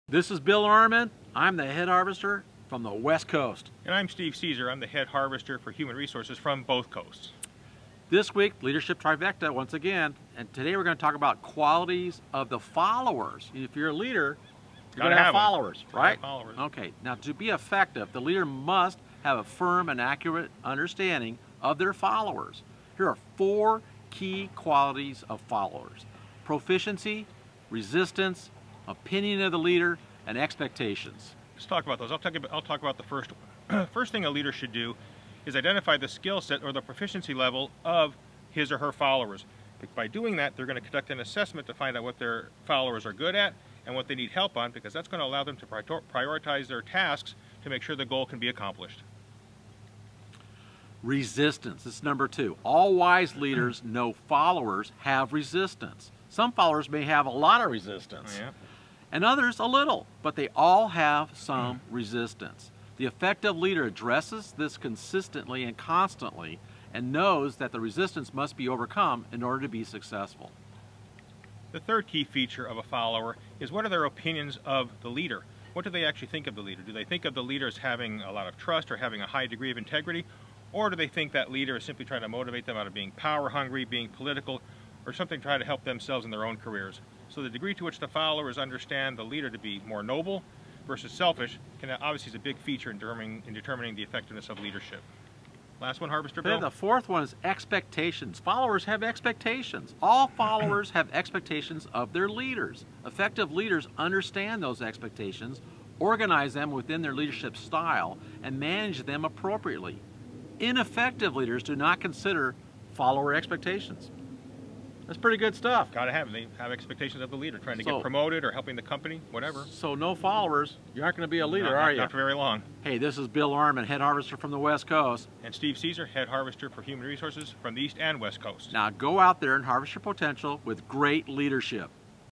Leaders Understand Their Followers - Do You?
- Leaders have followers.  Of course they do!
As a leader, to be effective, do you know the 4 key qualities of your followers?  Watch this 2 minute video as Harvesters Bill and Steve explain the 4 key qualities.  They are proficiencies, resistance, opinions, and expectations.  This session will be eye opening.